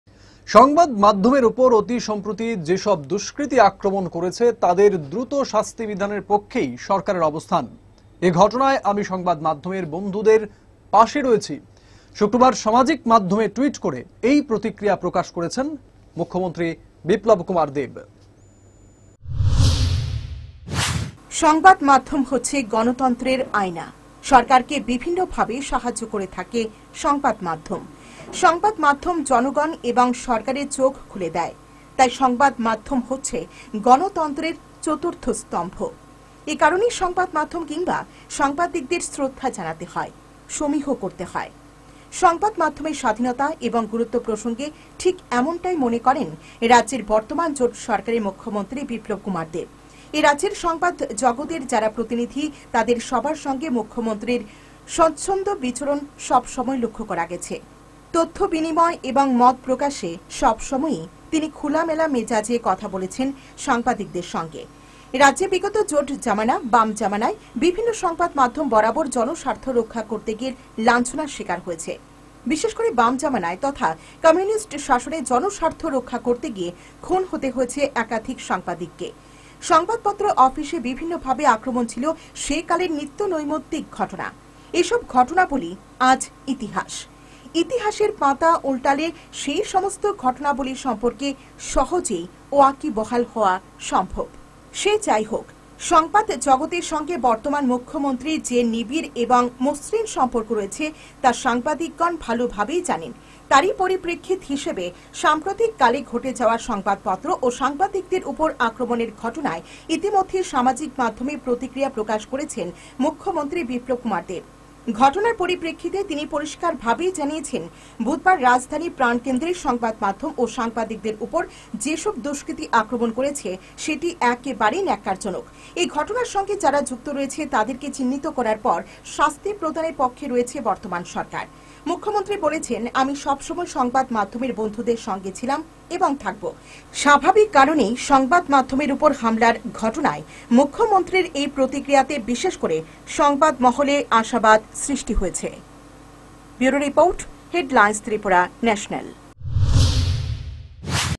সামাজিক মাধ্যমে টুইট করে এই প্রতিক্রিয়া প্রকাশ করেছেন মুখ্যমন্ত্রী বিপ্লব কুমার দেব
"সংবাদ মাধ্যমের ওপর অতিসম্প্রতি যেসব দুষ্কৃতী আক্রমণ করেছে তাদের দ্রুত শাস্তি বিধানের পক্ষেই সরকারের অবস্থান। এ ঘটনায় আমি সংবাদমাধ্যমের বন্ধুদের পাশে রয়েছি।" শুক্রবার সামাজিক মাধ্যমে টুইট করে এই প্রতিক্রিয়া প্রকাশ করেছেন মুখ্যমন্ত্রী বিপ্লব কুমার দেব।